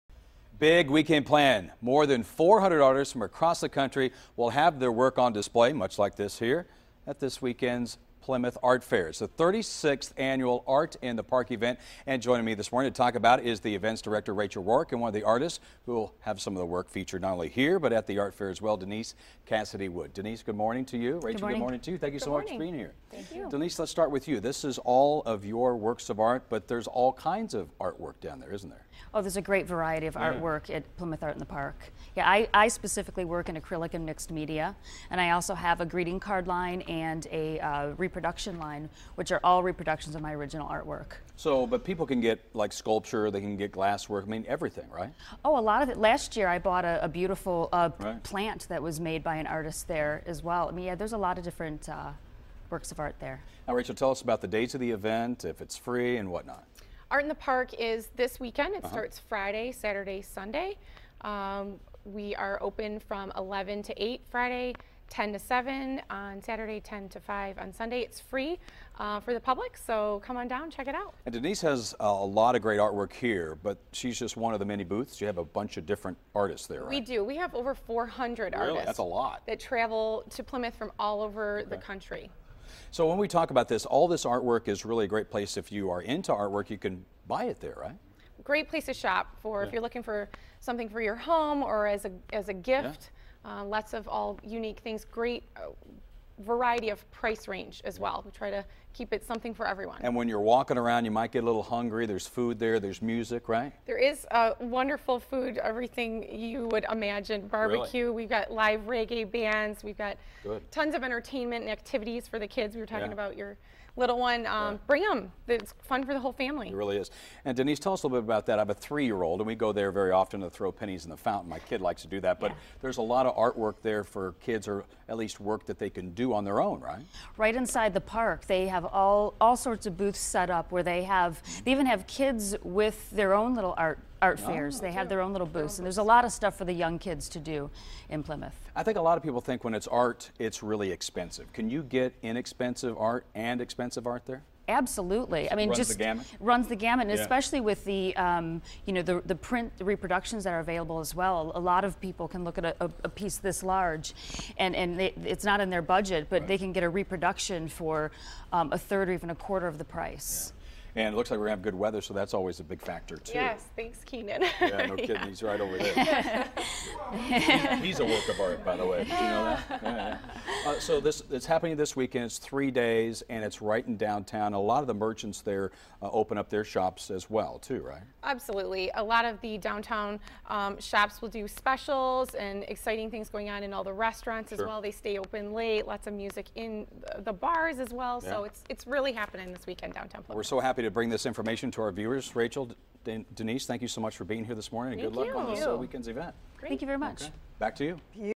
Art In The Park
Annual art fair returns to downtown Plymouth July 10-12